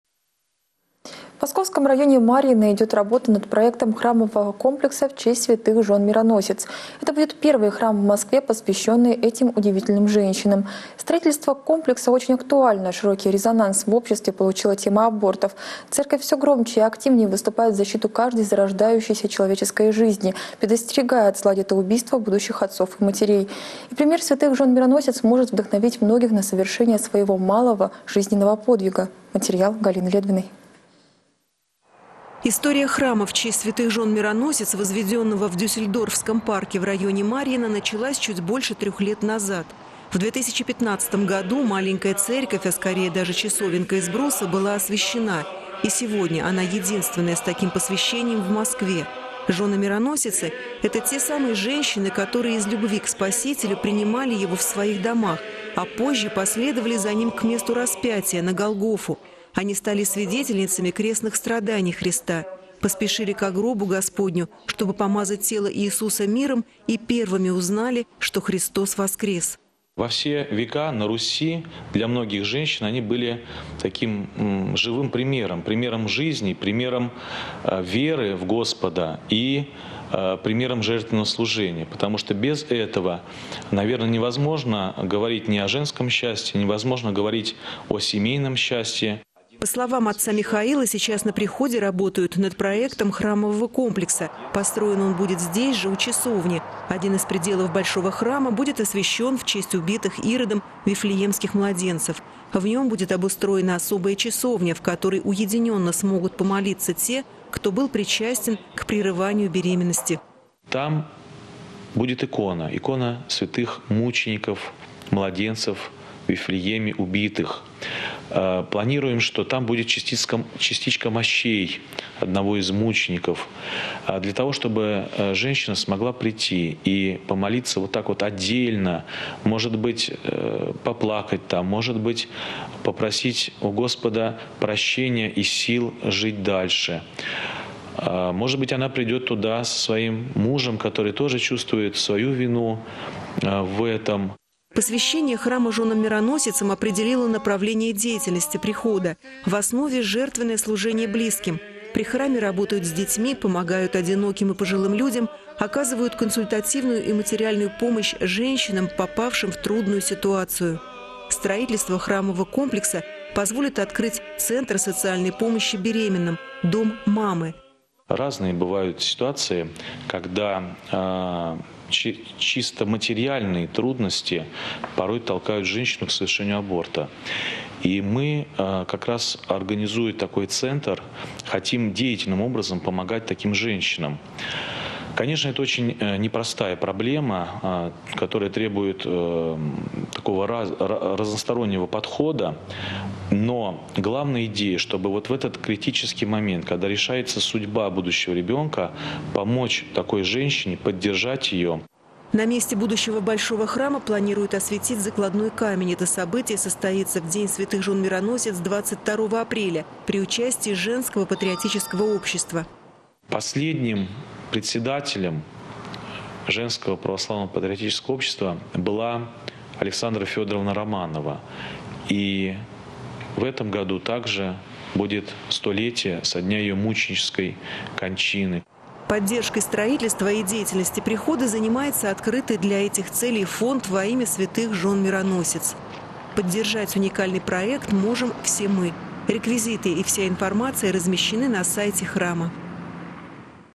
В Марьино идет работа над проектом храмового комплекса в честь святых Жен-Мироносиц
В Московском районе Марьино идет работа над проектом храмового комплекса в честь святых Жен-Мироносиц. Это будет первый храм в Москве, посвященный этим удивительным женщинам. Строительство комплекса очень актуально. Широкий резонанс в обществе получила тема абортов. Церковь все громче и активнее выступает в защиту каждой зарождающейся человеческой  жизни, предостерегая от зла детоубийства будущих отцов и матерей. И пример святых жен-мироносиц может вдохновить многих на совершение своего малого жизненного подвига.